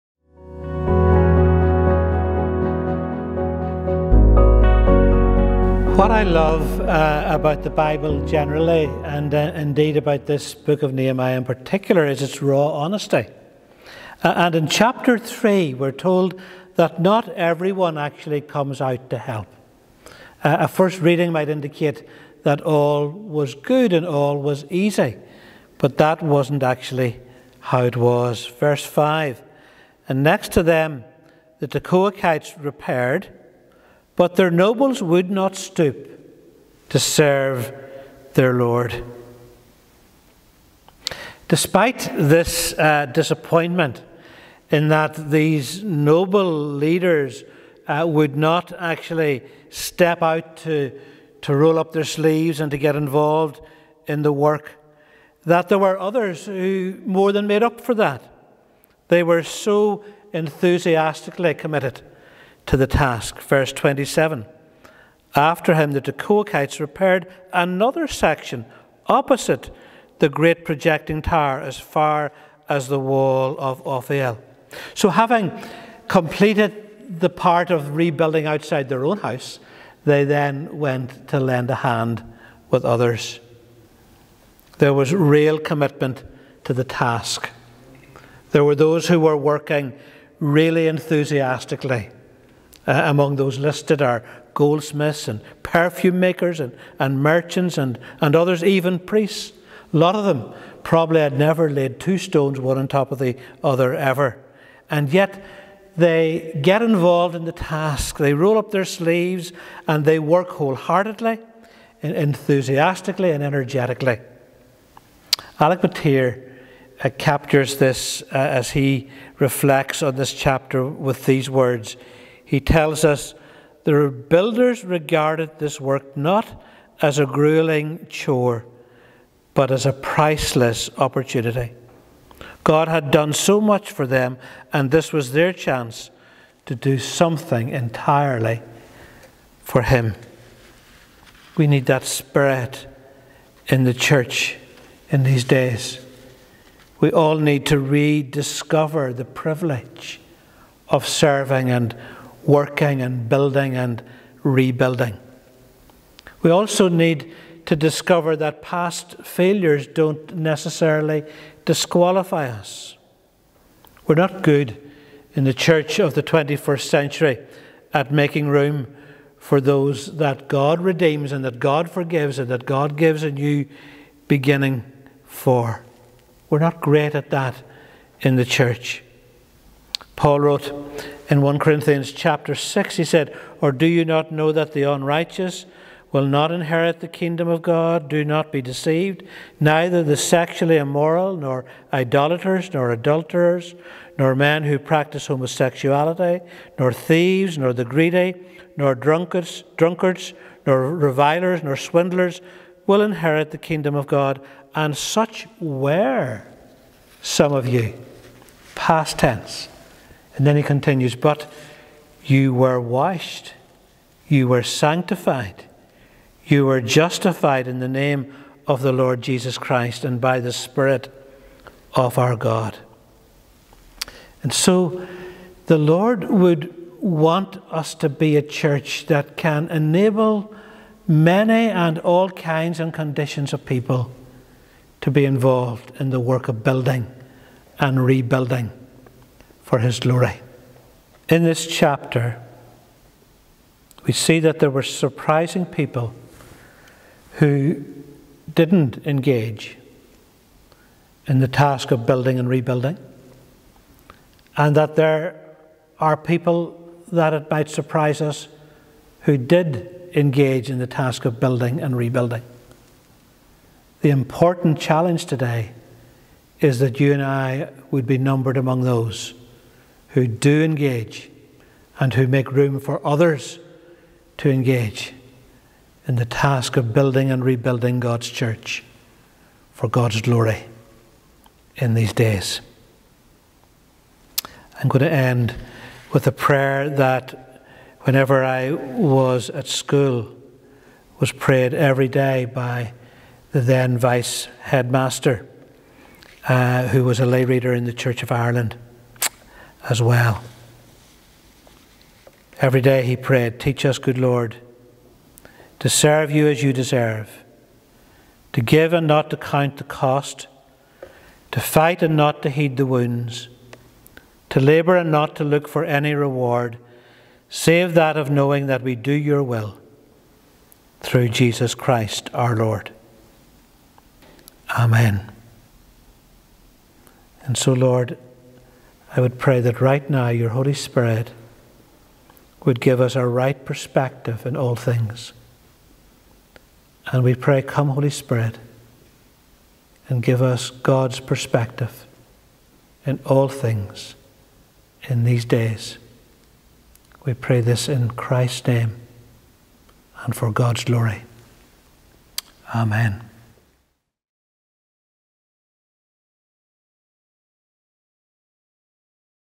Reflections in Nehemiah Day 10
With Bishop David McClay. Nehemiah 3. Everyone gets to participate.